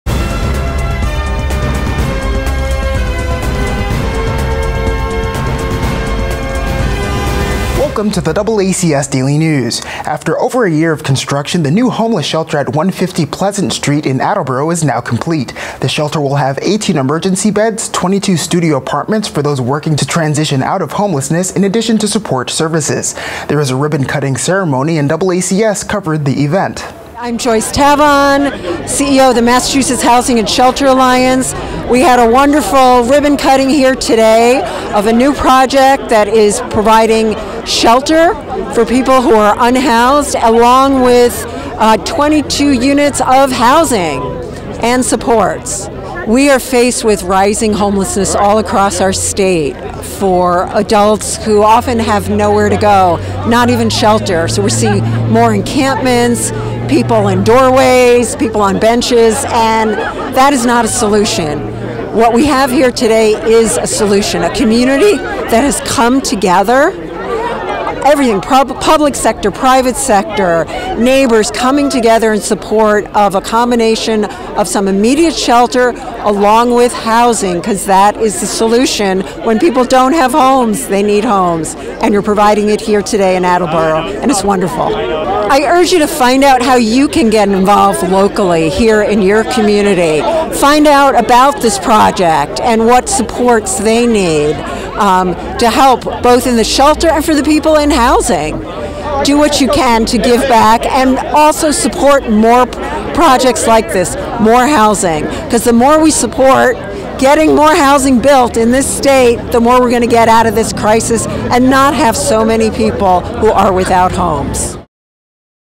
Attleboro Homeless Shelter Complete
After over a year of construction, a new homeless shelter on Pleasant Street is now complete. The shelter will have 18 emergency beds, 22 studio apartments for those working to transitioning out of homelessness, in addition to support services. There was a ribbon cutting ceremony - Learn more in the DoubleACS Daily News!